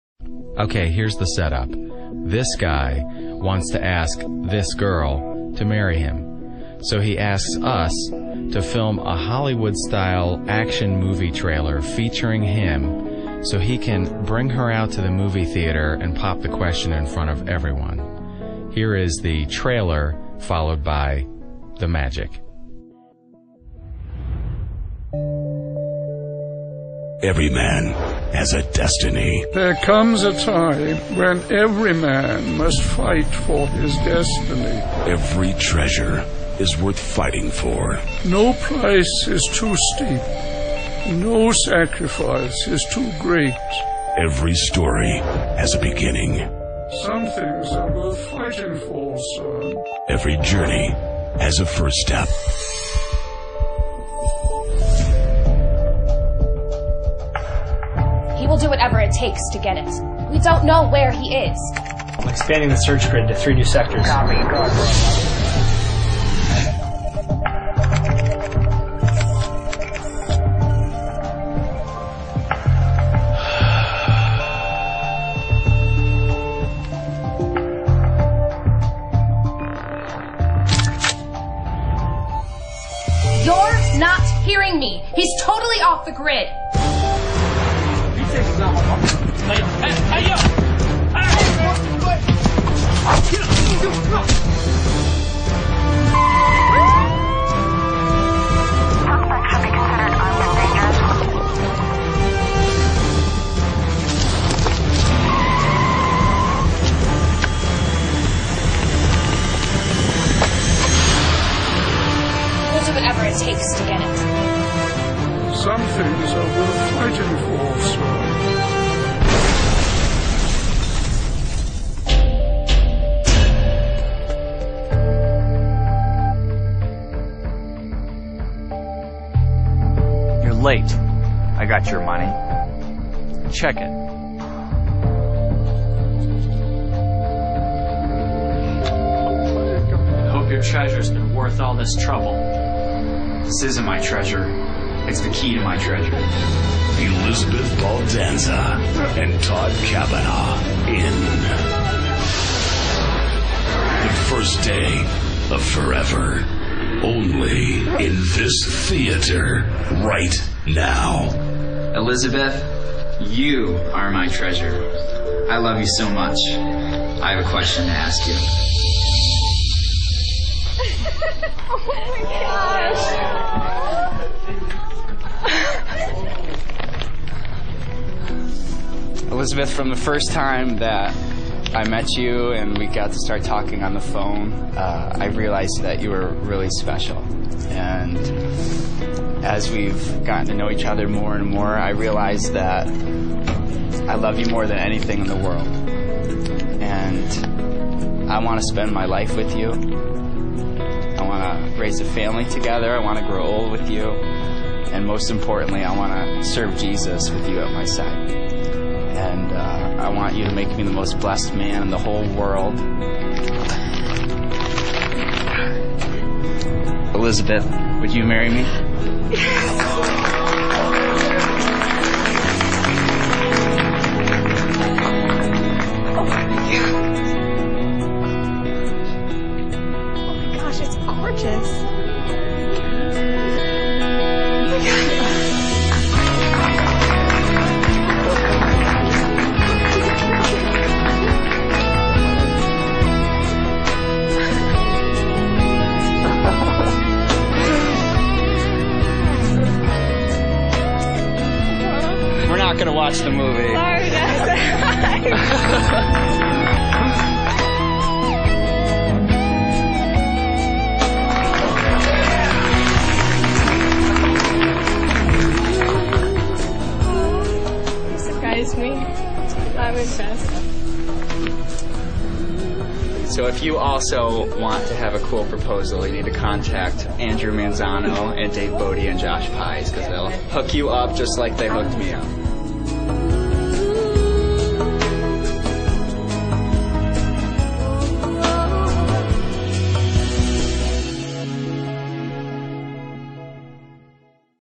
The BEST movie theater proposal EVER!!!
Todd Cavanaugh asks Elisabeth Baldanza (Miss New York, 2007)

Get a free IPAD 2 here in any color of your choice!
So simple anyone Can get one - Hurry Offer ends Soon!

to marry him with the help of his own action movie trailer. Filmed by The Sound Tank: ("I Fought For You") Director/ Editor - Andrew Manzano Camera/ SFX - Dave Bode Producer - Josh Pies-c47films.com, Movie Trailer voice - Charley Barnes (Contact him for your project: ) Special thanks to the Hyatt Regency Rochester.